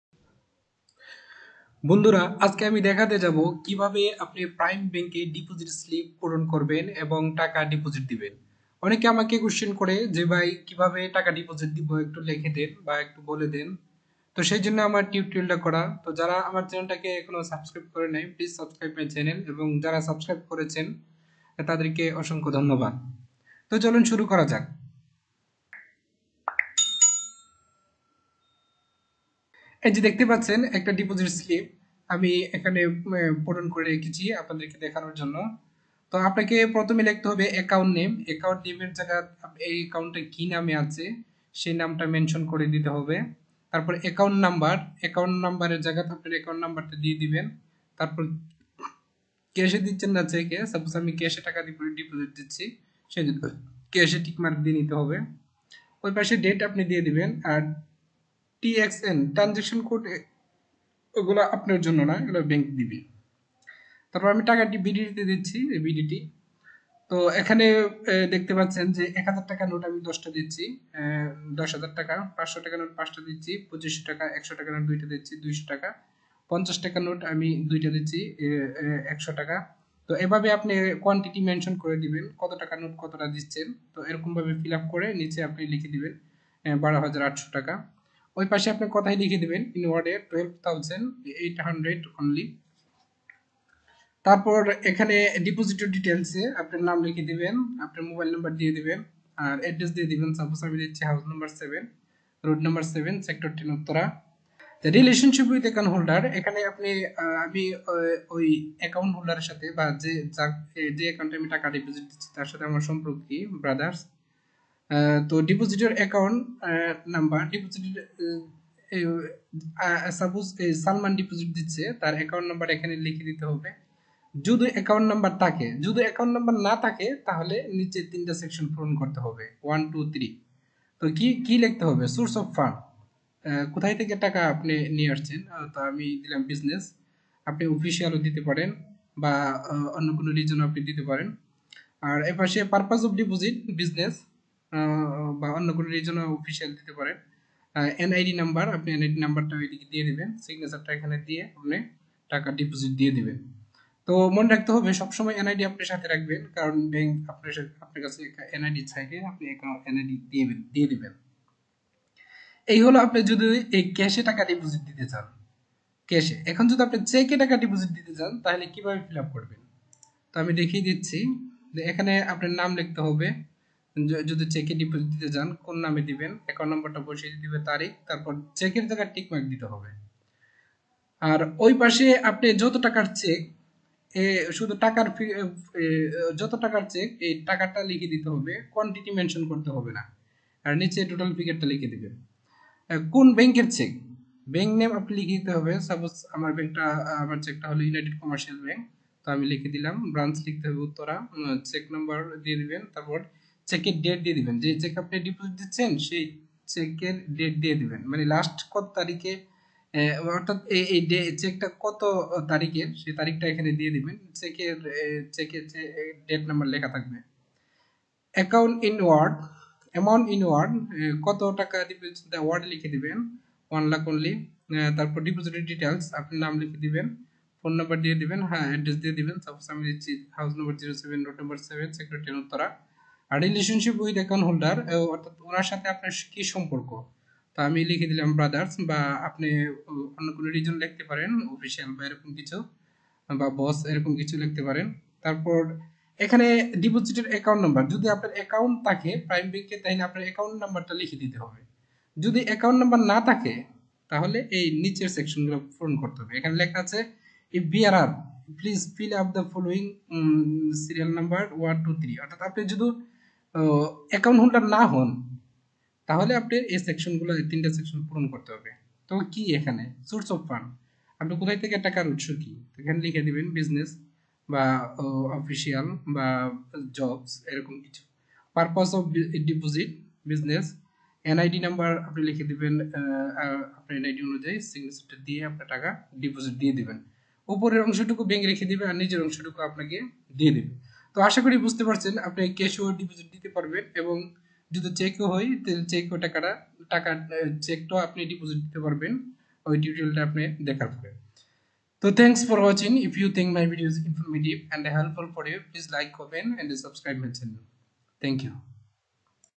How to fill up deposit form Prime Bank Limited-cheque deposit-cash deposit-online deposit
This special tutorial you learn how to fill up deposit form Prime Bank Limited. It's may be cheque/check deposit or cash deposit or online deposit. The idea about how to deposit prime bank and cash deposit offset of prime bank limited Bangladesh as well as the prime bank investment limited. Know how to fill up deposit form and deposit to prime bank system.